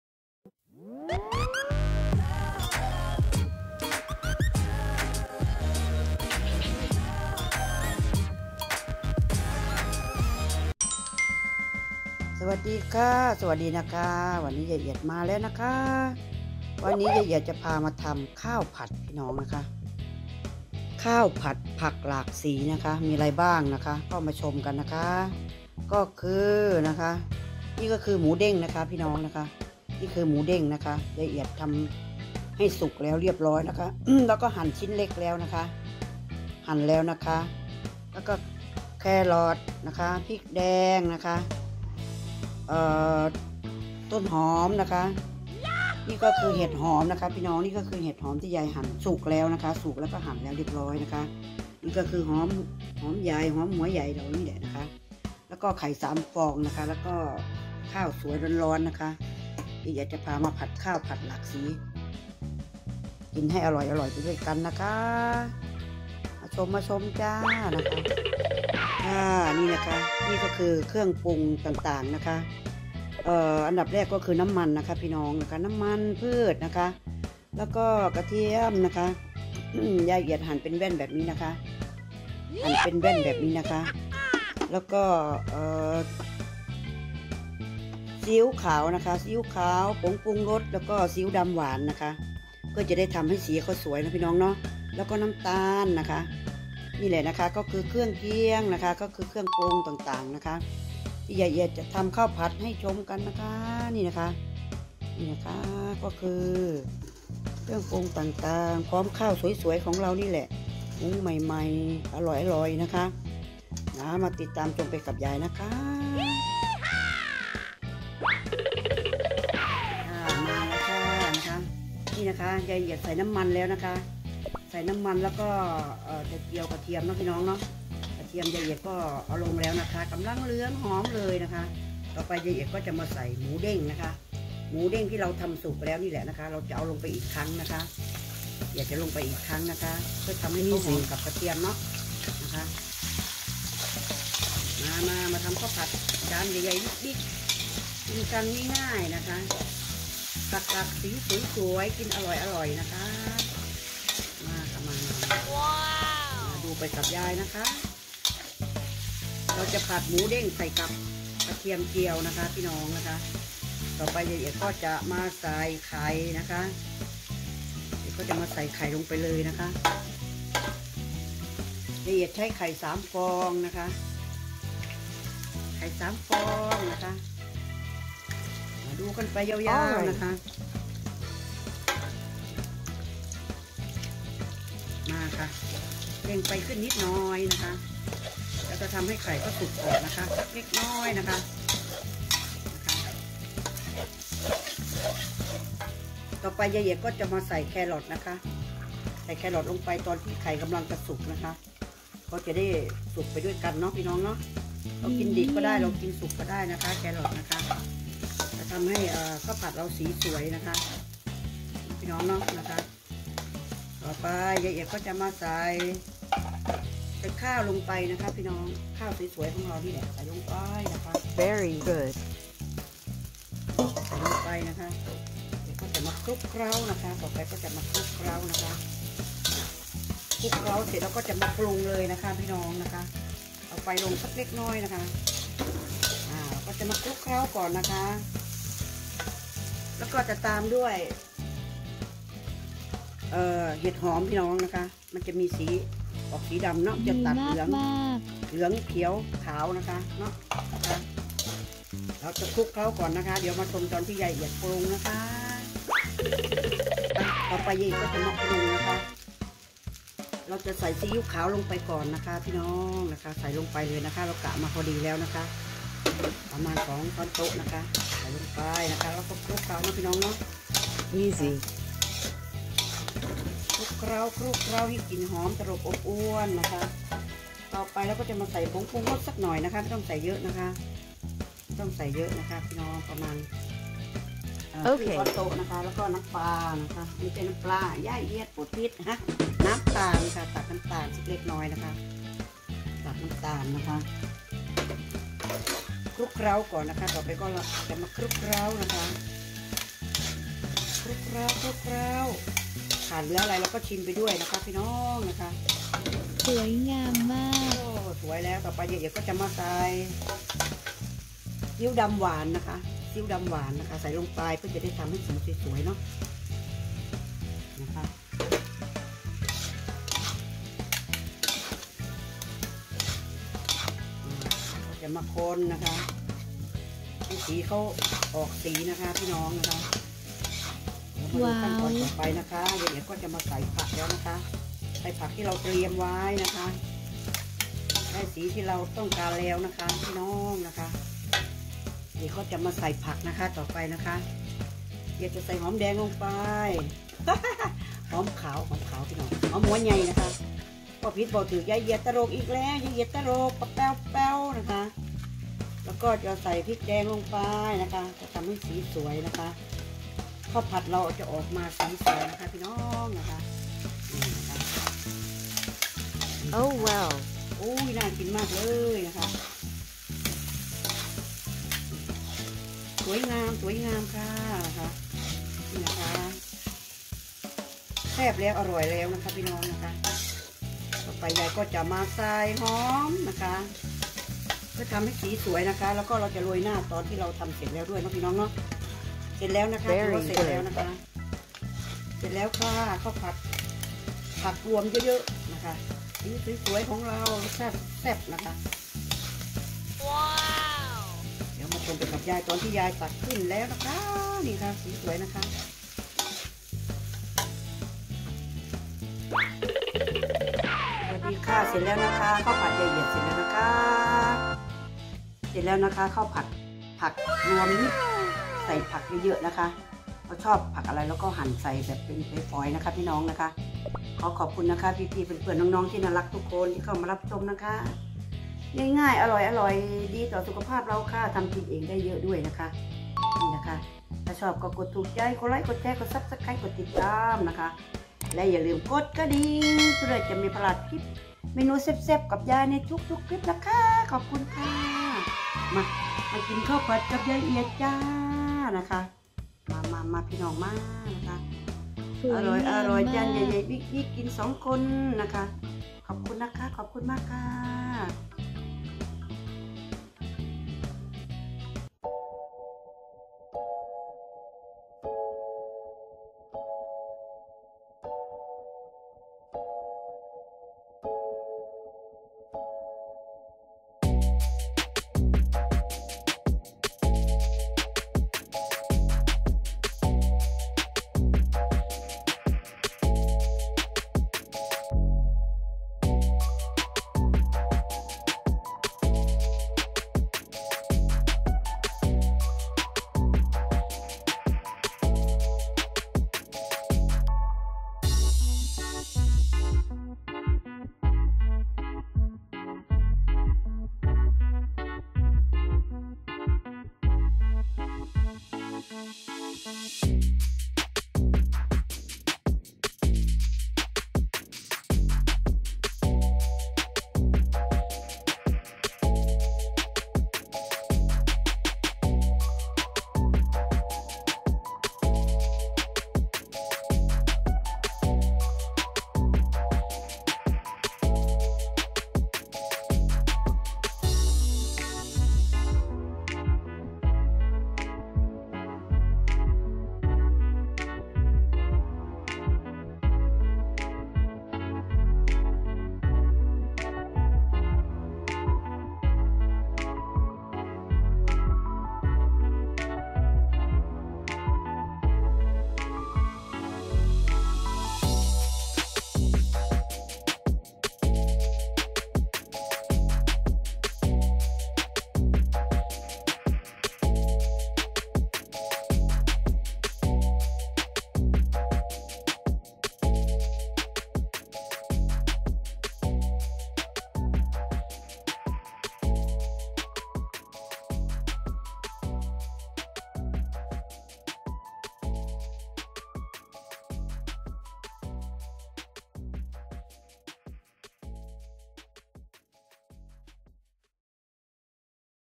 ข้าวผัดผักหลากสี สูตรง่ายๆ ใครก็ทำได้ เมนูอาหารอีสาน - อาหารว่าง ทําอาหารง่ายๆ
สวัสดีจ้า วันนี้ ยายเอียด พามาทำเมนู ข้าวผัดผักหลากสี  เป็นการทำ ทำข้าวผัด กับผักต่างๆที่มีสีแตกต่างกัน ทำให้ เมนูอาหารน่ากิน  กับสูตรง่ายๆ ใครก็ทำได้

นักพูดสร้างแรงบันดาลใจ ,
รายการอาหาร,
นักพูดให้กําลังใจ,
วิธีทําอาหารอีสาน,
ทำกินเอง,
นักพูด,